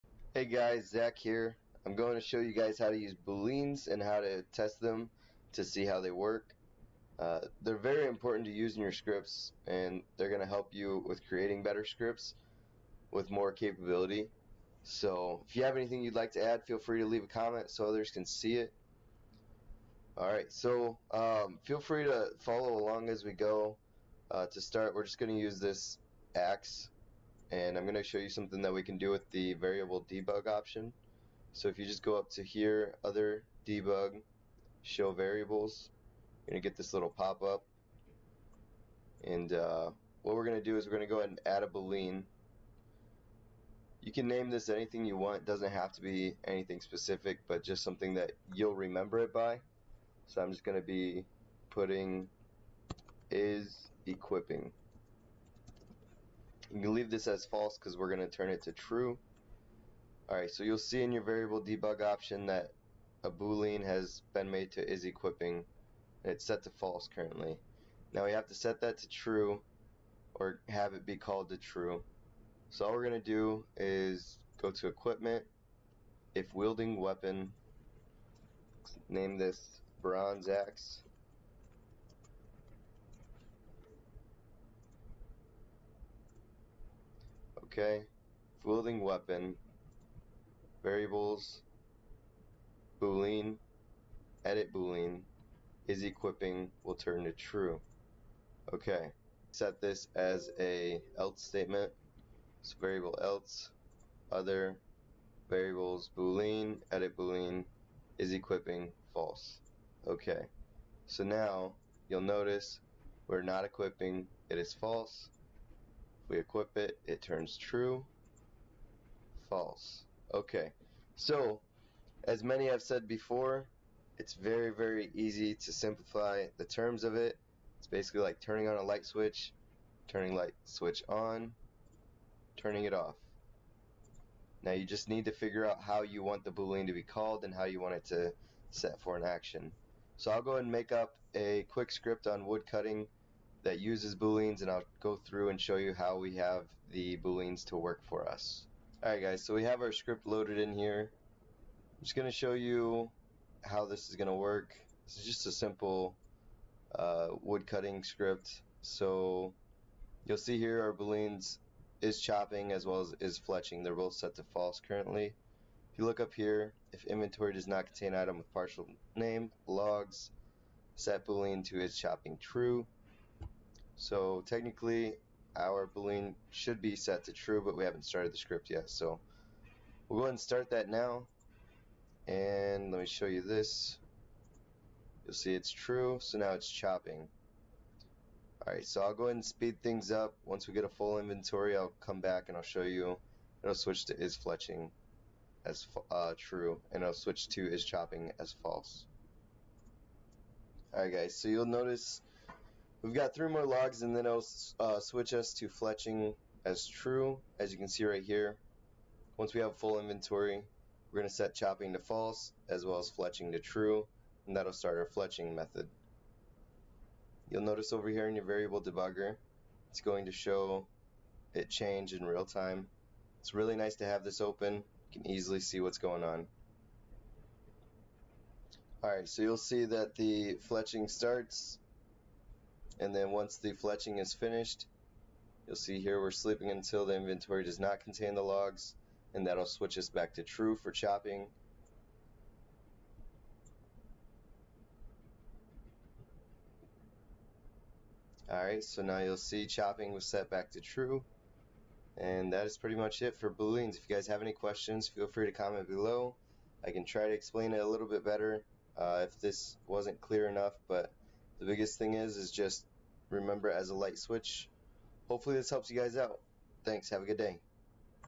Boolean Guide - Script Factory
Hey guys, this is a simple guide on how to use Booleans for Script Factory! Hope this helps!